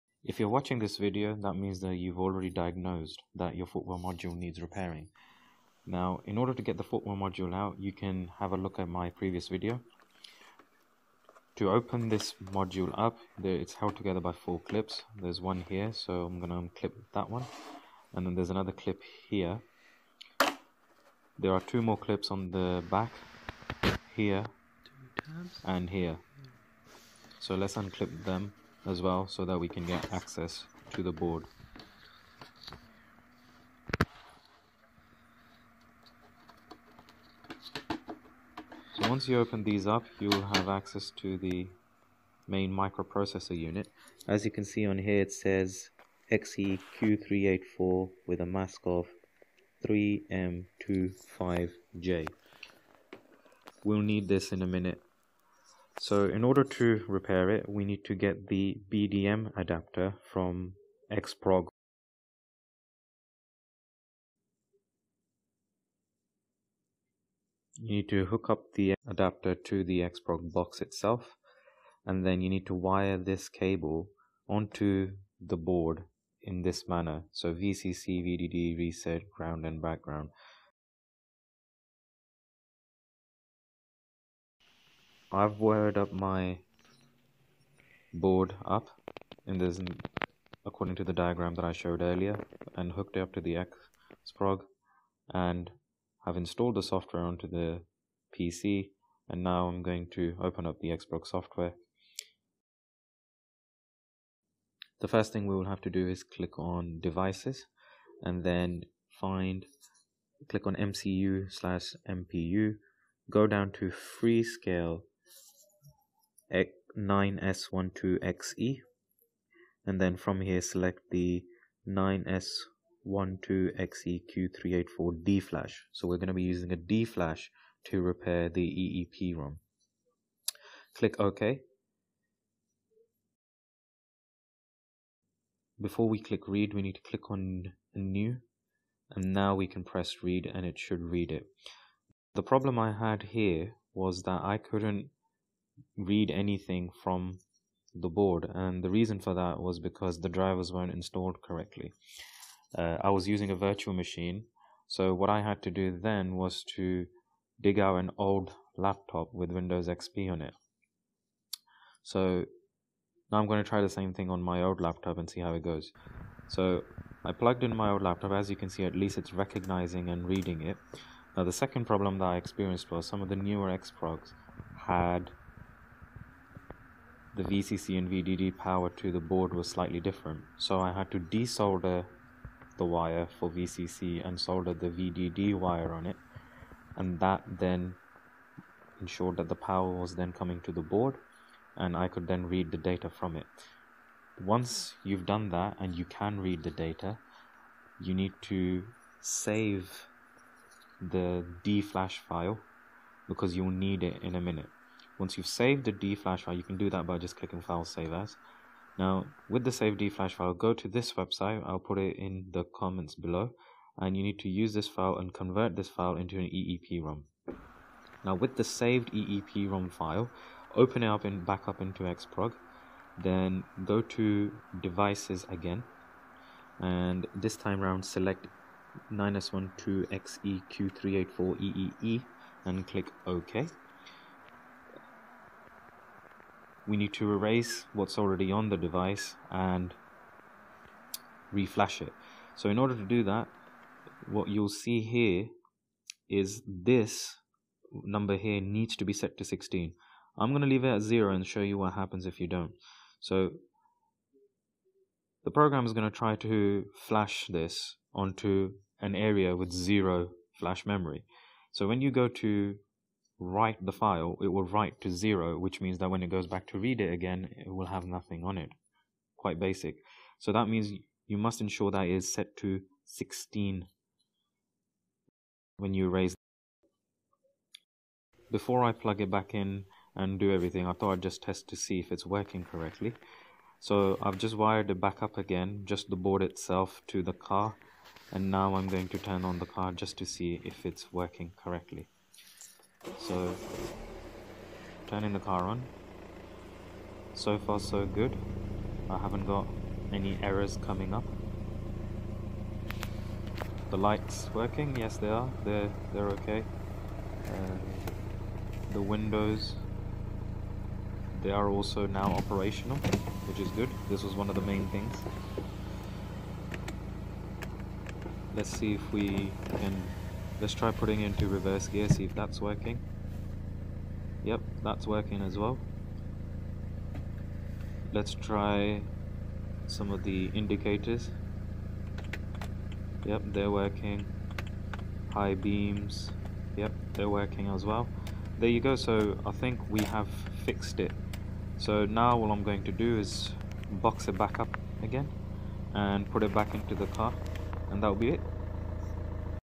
BMW how to repair foot-well module or lighting module, FRM FRM3, using xProg (or similar)
Video showing how to repair your existing module rather than purchase a new one for thousands. This method doesn't require you to reprogram the functionality as it is extracted from your existing setup.

You need to repair this if you are having problems doing a diagnostic test, mirrors, windows, central locking, reverse gear, high beams etc are not functioning correctly.

Microprocessor: Freescale MC9S12XEQ384 3M2J

Steps:
1) Solder the wires according to diagram in video using correct adaptor
2) Download the D-Flash from board
3) Convert D-Flash to EEEProm [Offline and online versions available]
4) Erase existing (corrupt) EEEProm on FRM3 (16 Sectors)
5) Write converted EEEProm
6) Verify(Check written file is same as saved file)
7) Test on car